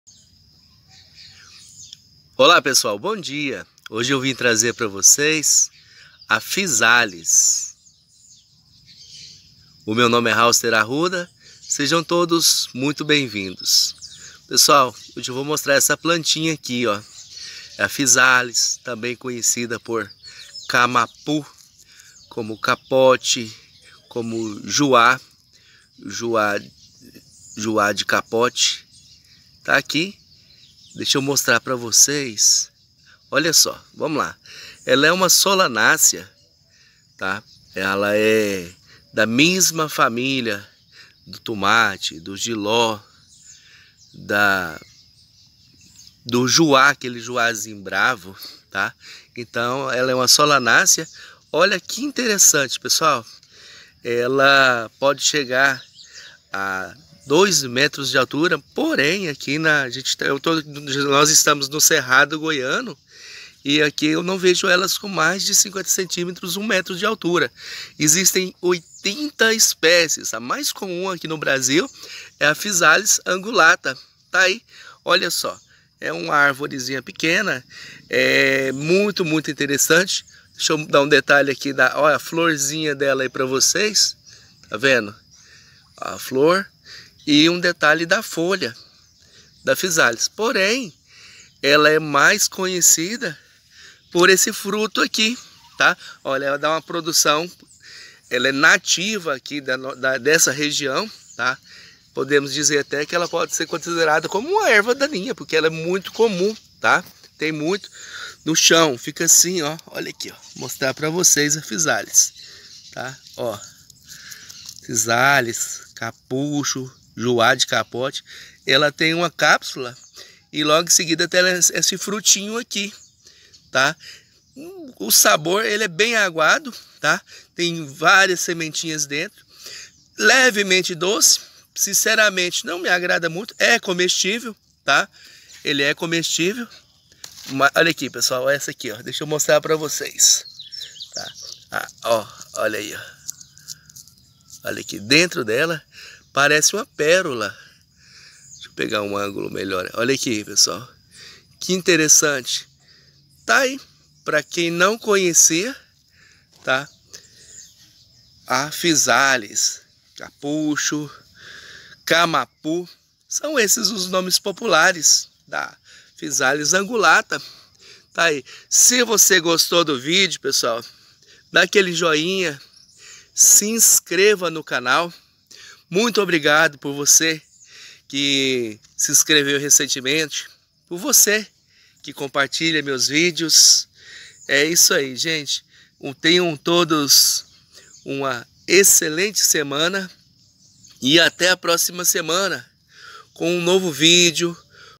Você conhece a Physalis?
Da família botânica solanacea
Cerca de 80 espécies em todo o mundo
A Colômbia é o maior produtor mundial
Pode chegar a 2 metros de altura
Plantio semelhante a cultura do tomate

Nomes populares:
Camapu
Phisalis
Fisalis
Capota
Capucho
Juá de capote
Juá poca
Saco de bode
Cereja de judeu
Alquequenge (Portugal)
Capucha (Açores e Cabo Verde)
Matipatipa (Sul da Angola)

Fonte: Wikipédia